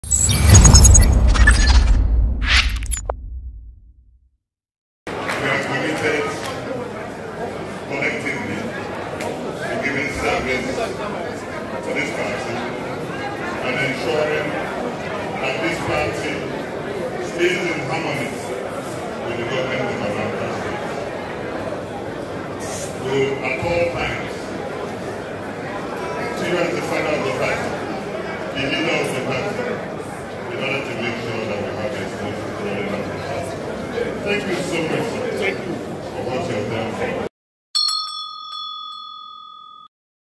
APGA Will Stay In Harmony - New APGA Anambra Chair, Obi-Okoye Vows
Hon. Ifeatu Obi-Okoye, Anambra State APGA chairman in an acceptance speech after his election at the congress held at the Professor Dora Akunyili Women Development Center, Awka on Tuesday May 23, 2023